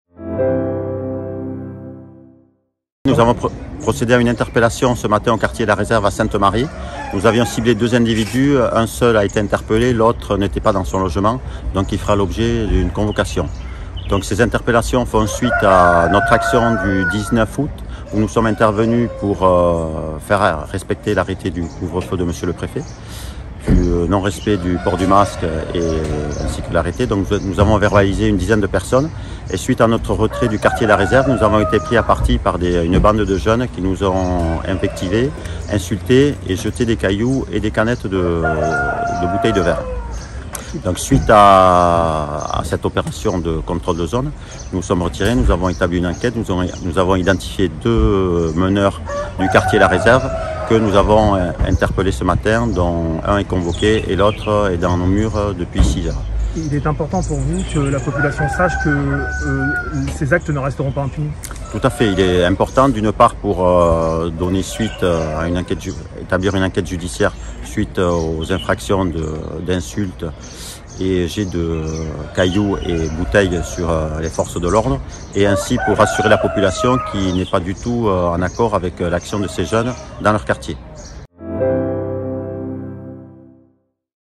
Opération de Gendarmerie
Ce mardi matin à l'aube, les militaires ont mené une opération d'interpellation dans le quartier de la Réserve sur la commune de Sainte-Marie